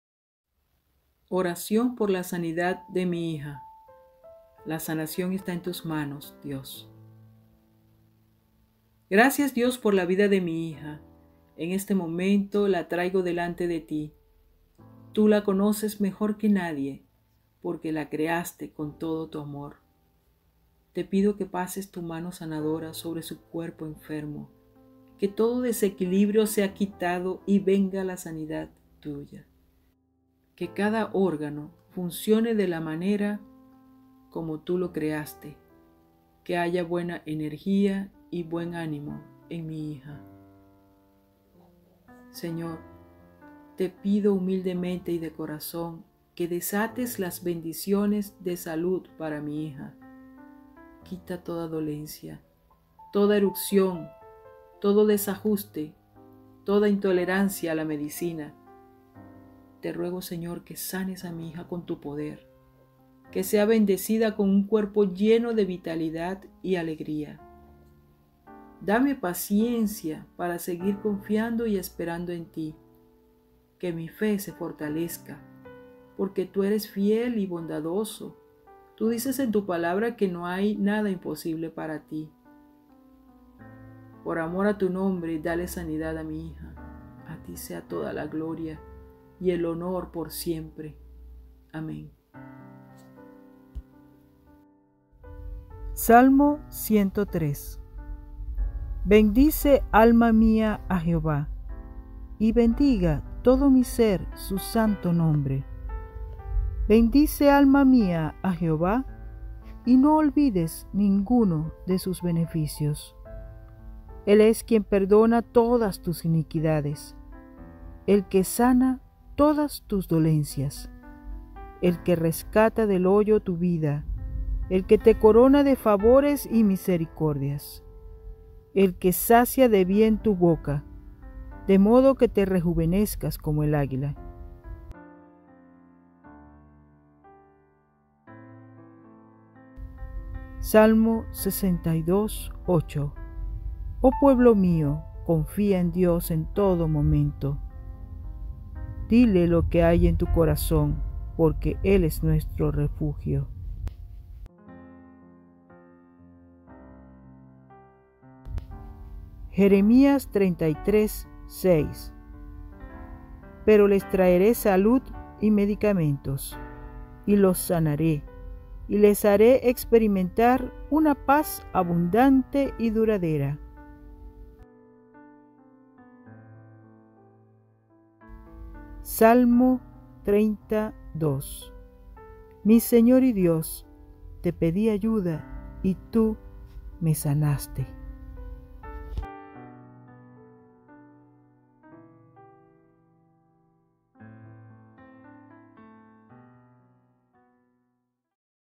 🙏🏻 Oración por la Sanidad de mi Hija enferma. Oración de Sanación de una Madre por su Hija 😷
Esta oración corta por mi hija enferma es poderosa, le pedimos a Dios que puede hacer el milagro de sanarla de toda dolencia, de todo virus, covid, infección, inflamación de sus órganos o de todo tipo de dolor.  La sanación es posible porque Dios extiende su mano de poder y sanidad, con su divino poder mi hija querida será sanada.  Al final de la oración hay varios Salmos de Sanidad y otros versículos para los enfermos.   Puedes hacer las otras oraciones que están en el blog por la sanidad de una hija o por otros seres queridos enfermos.